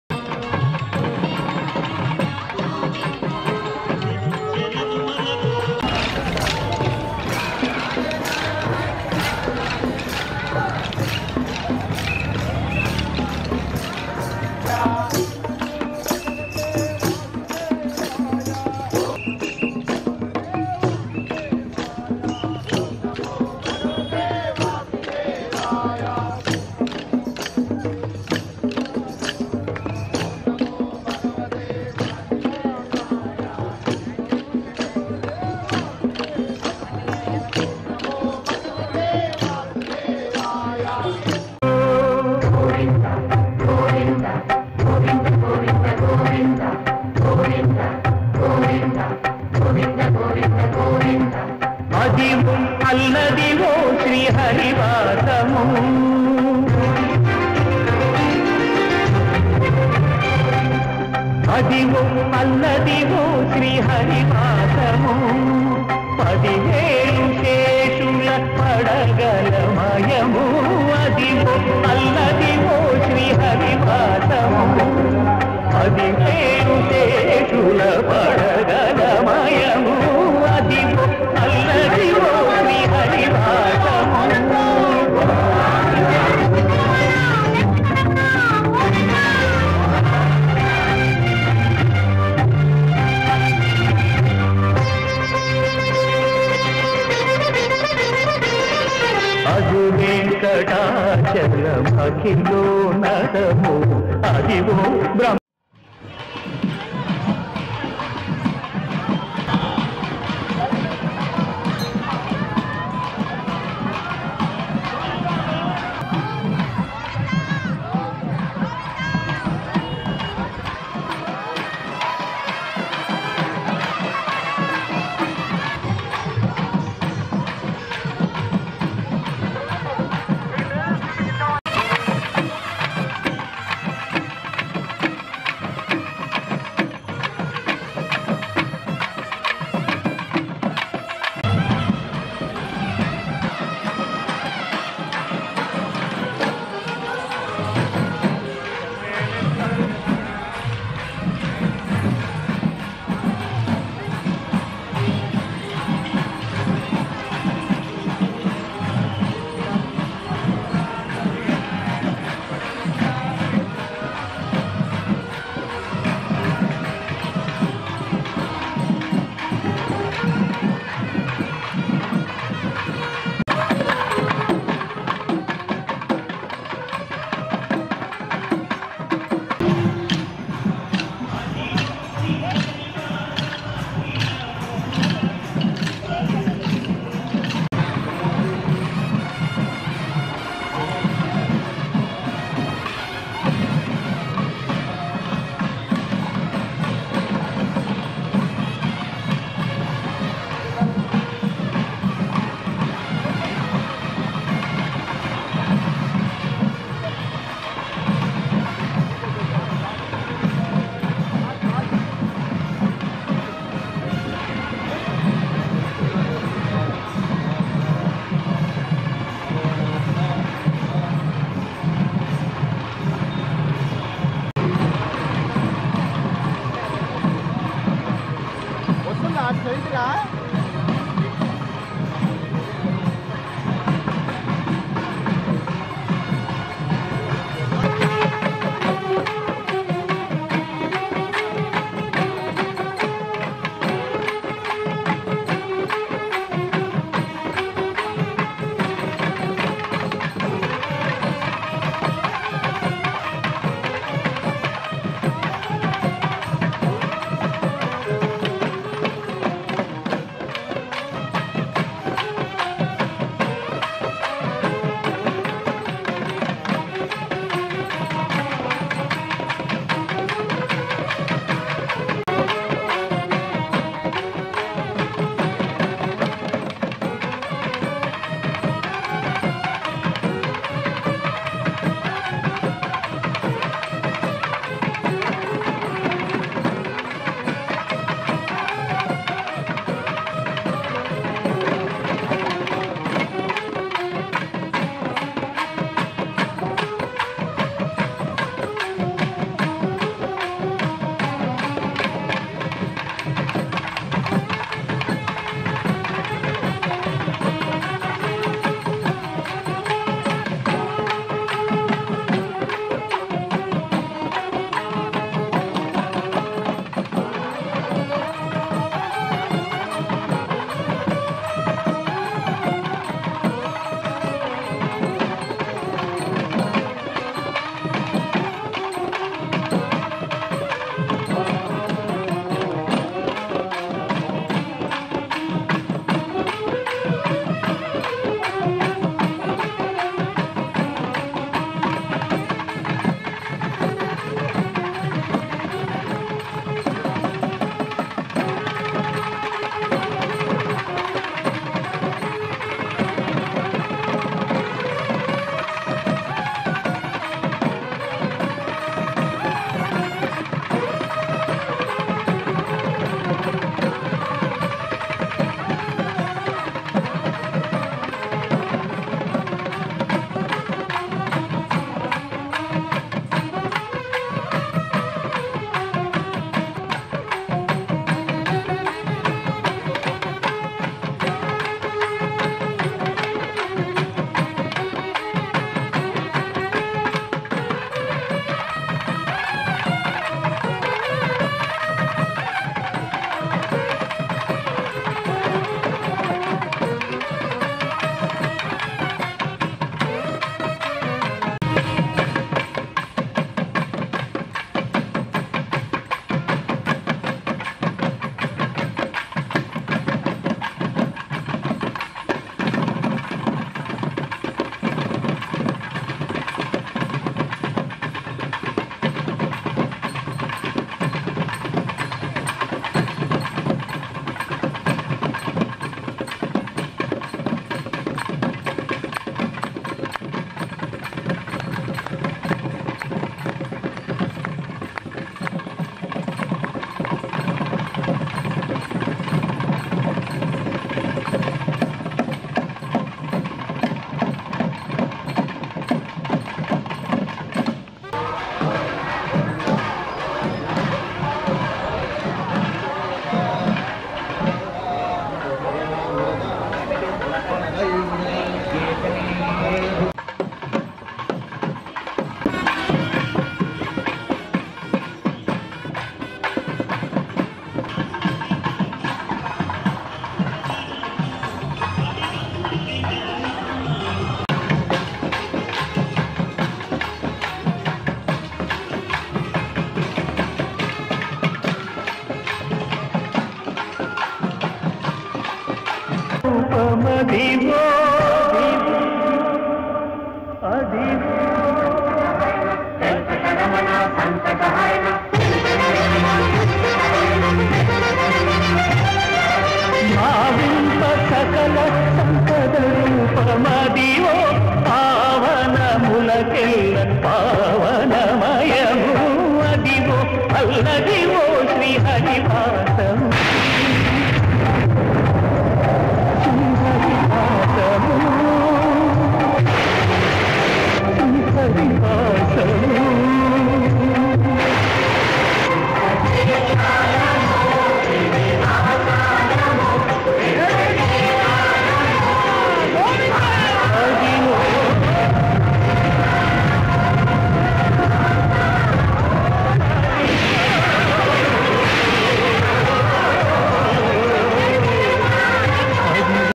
𝐑𝐚𝐭𝐡𝐨𝐭𝐬𝐚𝐯𝐚 𝐓𝐢𝐫𝐮𝐩𝐚𝐭𝐡𝐢 𝐁𝐚𝐥𝐚𝐣𝐢 (𝐓𝐓𝐃) 𝟐𝟎𝟐𝟒
The 19th Rathotsava celebrations of TTD, Liberty, Hyderabad, on 16 June 2024